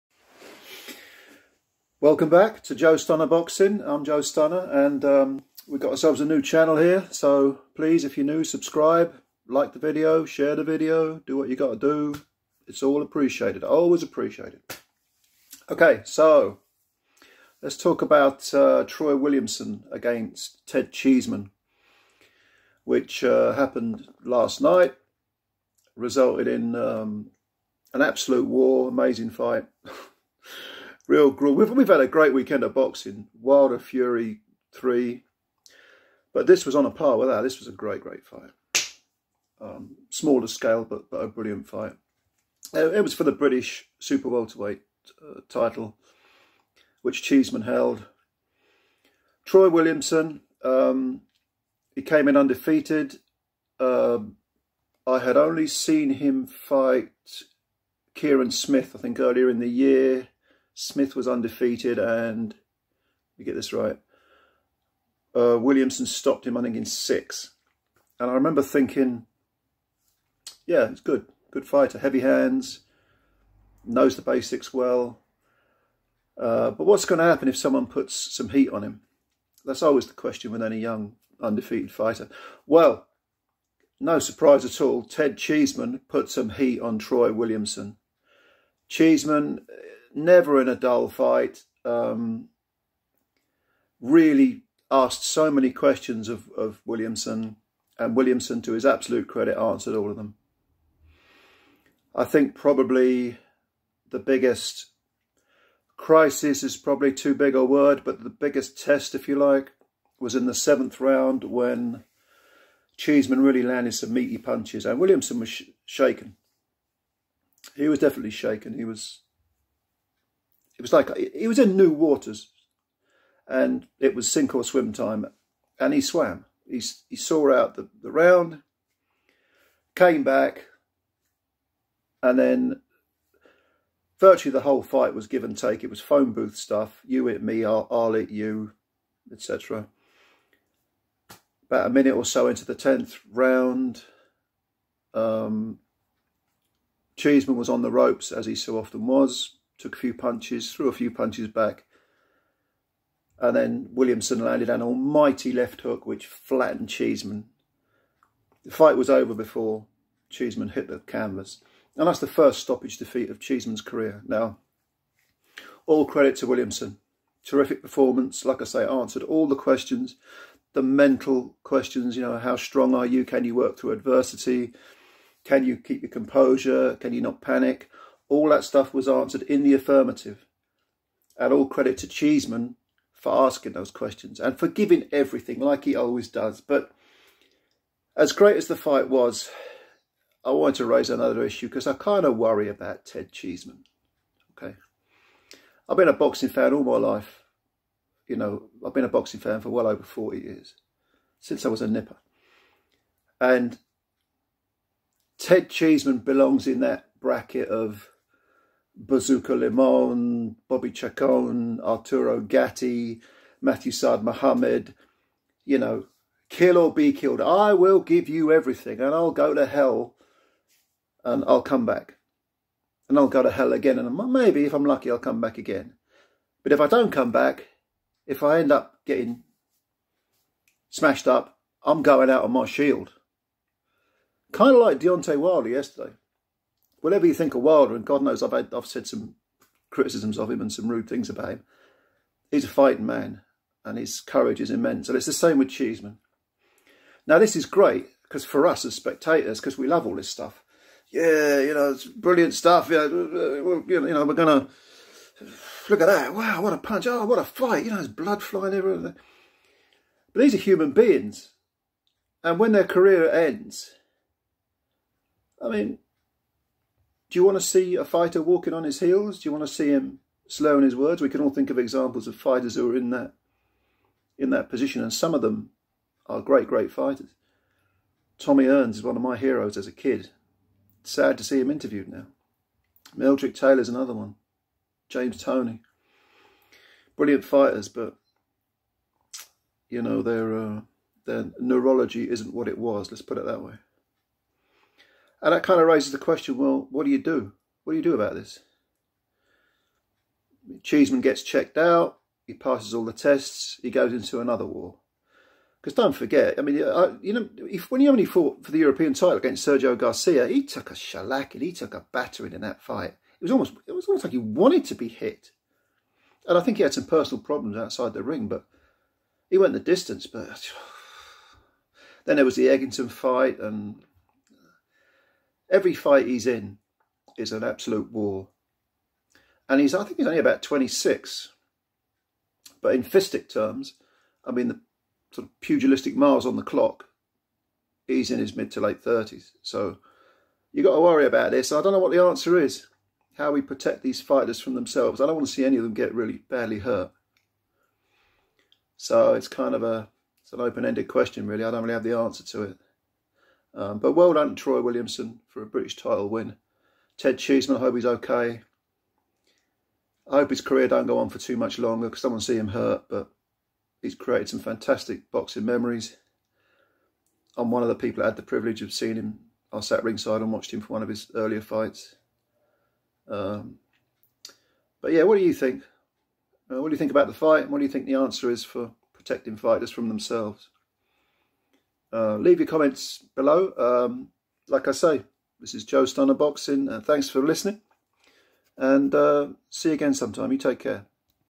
Troy Williamson, Ted Cheeseman, and what to do about saving boxers from themselves.
Ted Cheeseman was once again involved in a balls-to-the-wall thriller, this time against Troy Williamson.  Big credit to Williamson for the win, but is it time for Cheeseman to pack it in?  The blitzkrieg beatings are piling up and maybe he needs to be saved from himself but how do we do that where long, gruelling careers are concerned?